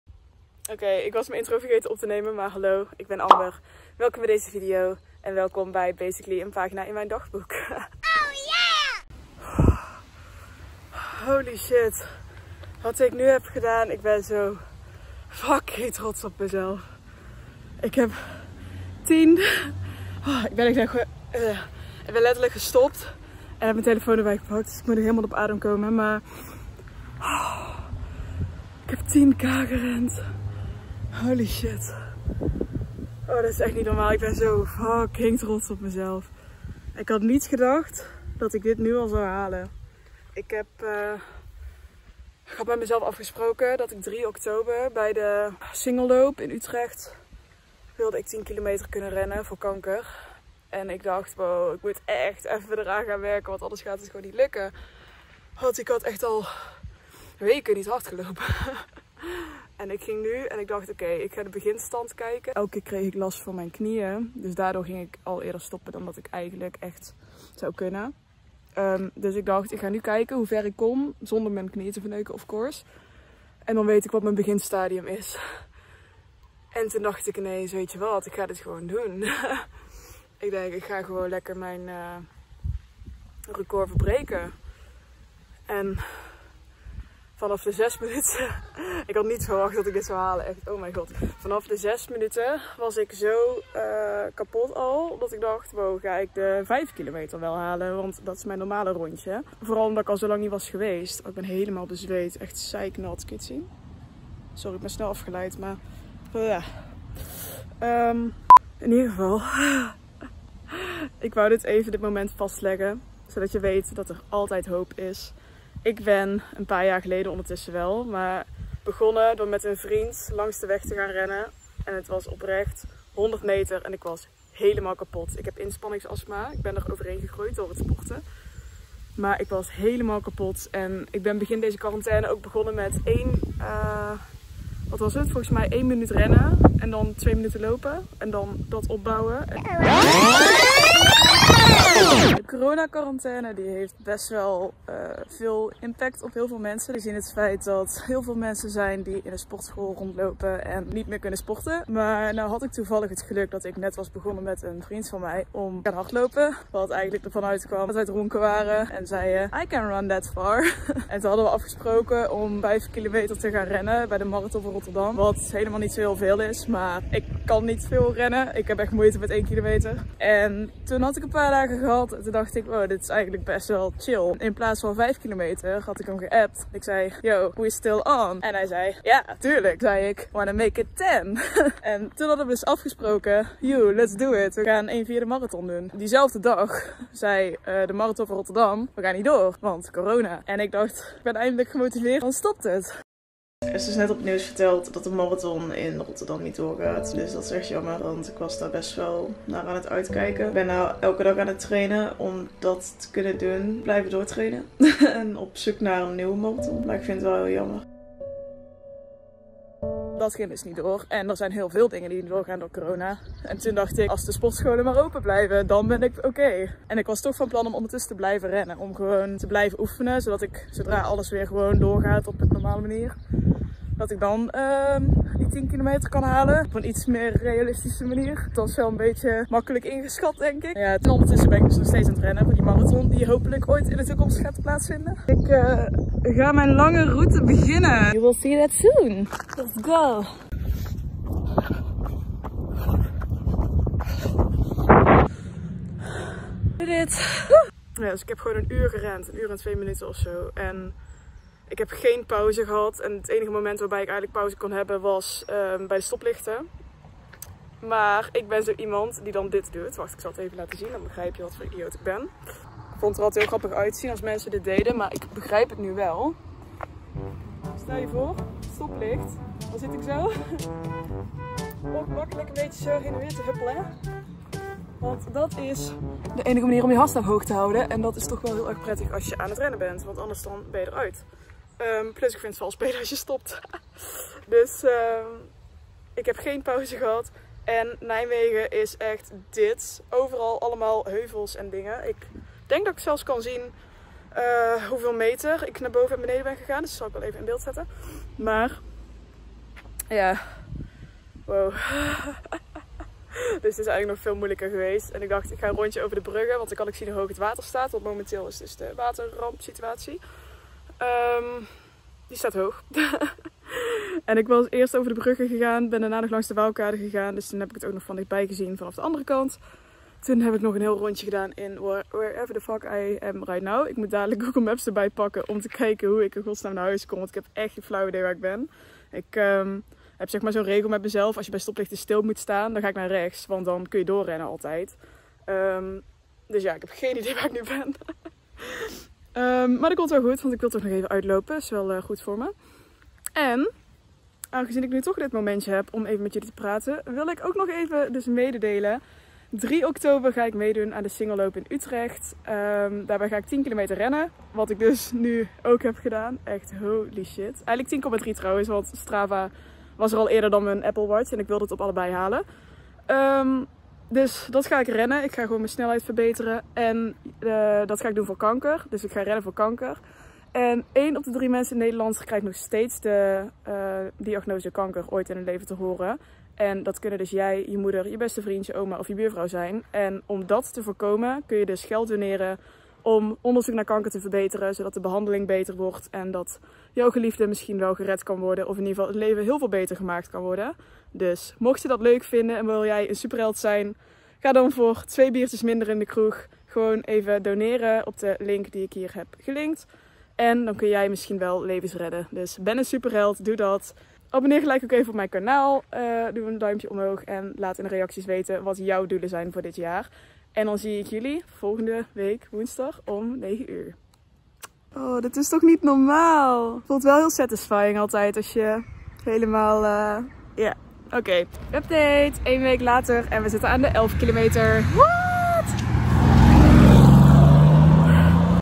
Ik Ga Rennen Tegen Kanker! (en eindelijk de 10 km gehaald whoopwhooop)
Wie mij vanaf het begin volgt weet dat ik was beginnen met hardlopen (voornamelijk ook door de corona situatie). Na 1.5 jaar zwoegen is het me gelukt om 10 km aan een stuk te rennen, en dat emotionele moment wilde ik even vastleggen. Ook heb ik besloten om te gaan rennen tegen kanker! Niet omdat ik iemand ken die dit nodig heeft, maar omdat ik graag mijn steentje wil bijdragen in deze maatschappij. Wil jij dat ook doen? Klik dan vooral even op het linkje hieronder :)

Ik heb zelf het 0 tot 5 km schema gebruikt van runnersworld, en die vind ik zeker aan te raden mocht je zelf willen beginnen met rennen! (no spon)

Volg me vooral eventjes op instagram!

Ik post iedere woensdag om 9:00 dus tot de volgende!!

Liefs, Amber.